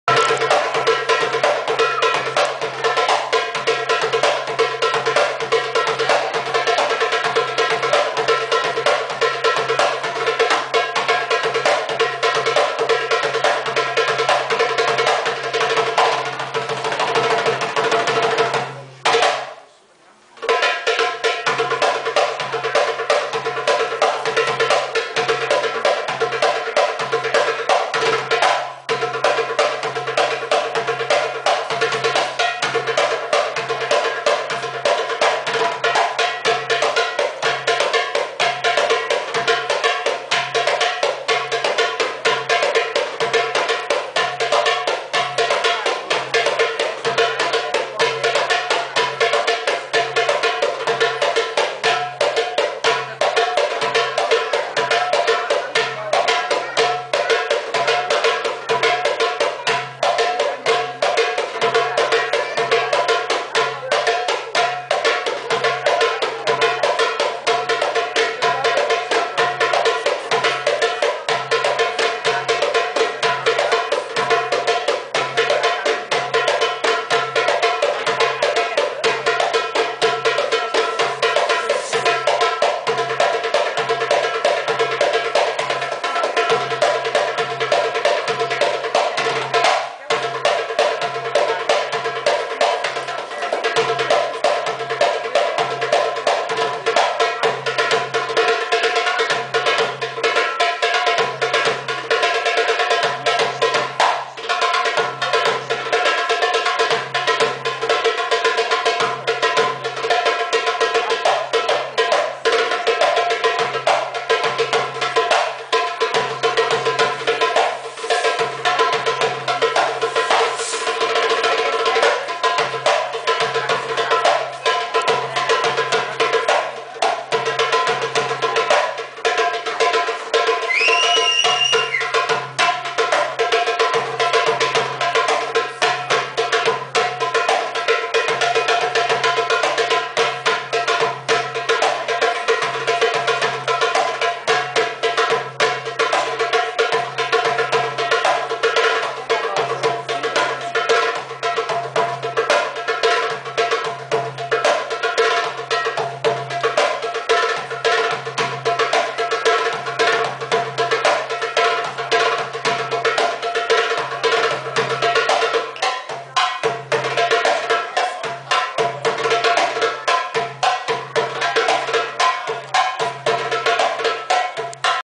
Sabrina Marchezini e Amanda - derback ao vivo - terceiro festival Paula Soares bh/MG
Derback ao vivo com as bailarinas Sabrina Marchezini, Amanda, Renem, Josiane  e Talita Reis. Participação de Luciano (derbackista) no terceiro festival do studio Paula Soares bh/MG.